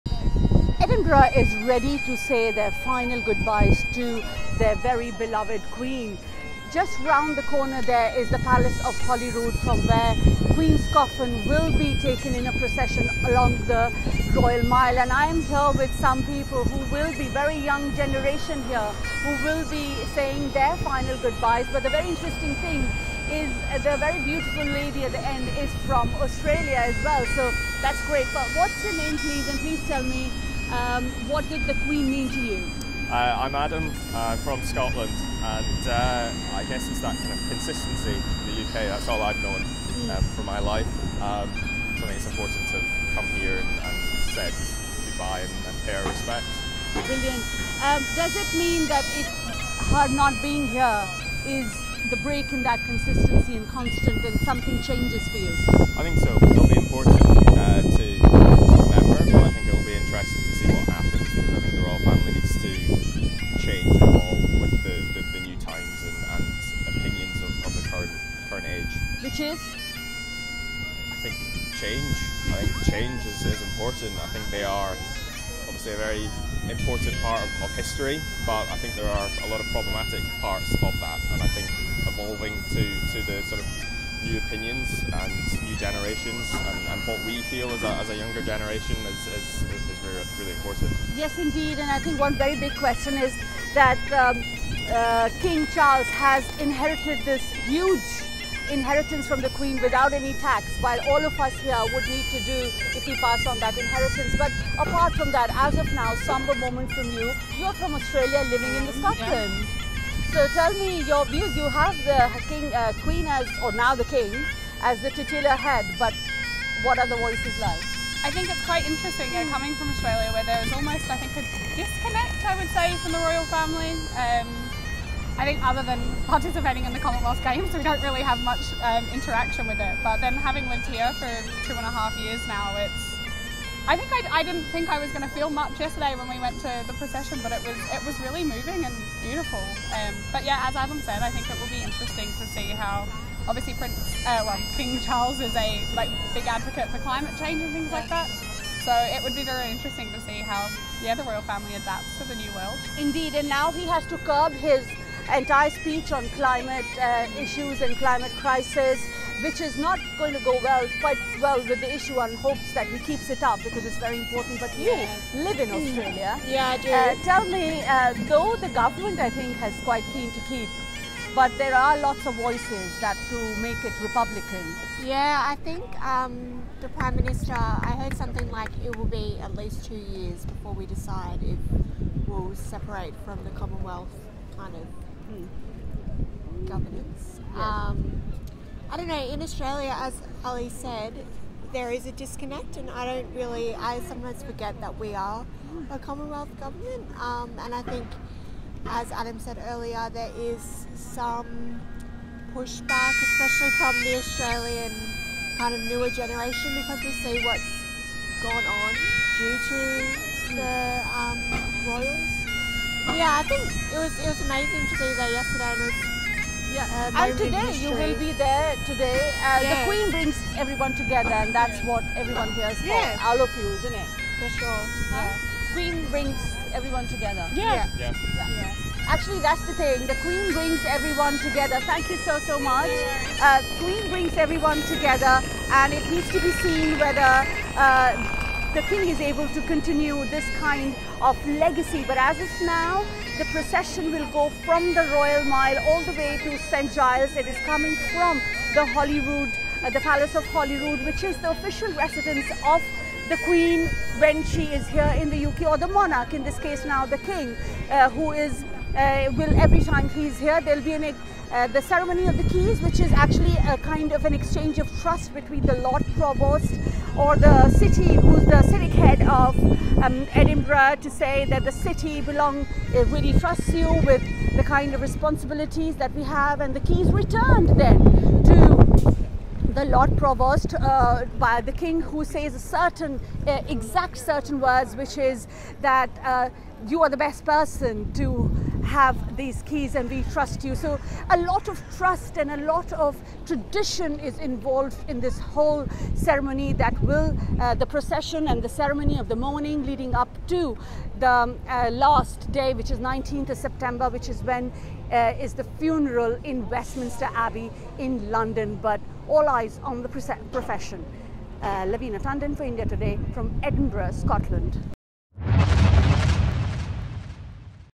Edinburgh Ground Report: Citizens Gather To Pay Their Last Respects To Queen Elizabeth
Queen Elizabeth News: Queen Elizabeth’s coffin, from the ballroom of Balmoral castle near Ballater village in the East of Scotland, will undertake its first lapse of the journey through the twin adjacent towns Aberdeen and Dundee on September 11 to reach Holyroodhouse Edinburgh the same day.

At about 11 am on September 12, a special procession will be held in which the king and the members of the royal family also take part. The coffin will make its way along the long mile to St. Giles Cathedral where it will be ‘lying in rest’ for people to pay respects.